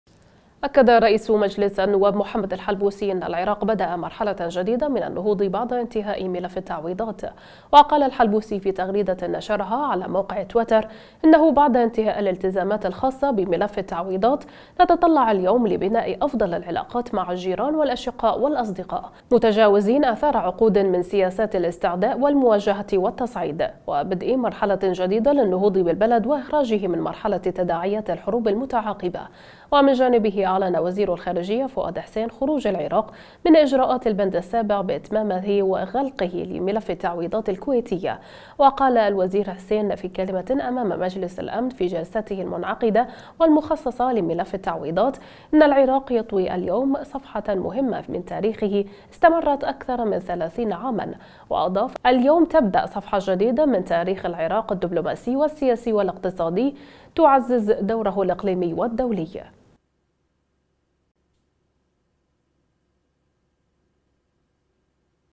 الحلبوسي: نتطلَّع لبناء أفضل العلاقات مع الجيران والأشقاء
أكد رئيس مجلس النواب محمد الحلبوسي أن العراق بدأ مرحلة جديدة من النهوض بعد انتهاء ملف التعويضيات. وقال الحلبوسي في تغريدة نشرها على موقع تويتر" انه بعد انتهاء الالتزامات الخاصة بملف التعويضات، نتطلَّع اليوم لبناء أفضل العلاقات مع الجيران والأشقاء والأصدقاء، متجاوزين آثار عقودٍ من سياسات الاستعداء والمواجهة والتصعيد، وبدء مرحلة جديدة للنهوض بالبلد وإخراجه من مرحلة تداعيات الحروب المتعاقبة". من جانبه أعلن وزير الخارجية فؤاد حسين، خروج العراق من إجراءات البند السابع، بإتمامه وغلقه لملف التعويضات الكويتية.  وقال الوزير حسين في كلمة أمام مجلس الأمن في جلسته المنعقدة والمخصصة لملف التعويضات إن "العراق يطوي اليوم صفحة مُهمة من تأريخه استمرت أكثر من ثلاثين عاماً".  وأضاف "اليوم تبدأ صفحة جديدة من تأريخ العراق الدبلوماسي والسياسي والاقتصادي تُعزز دوره الإقليمي والدولي".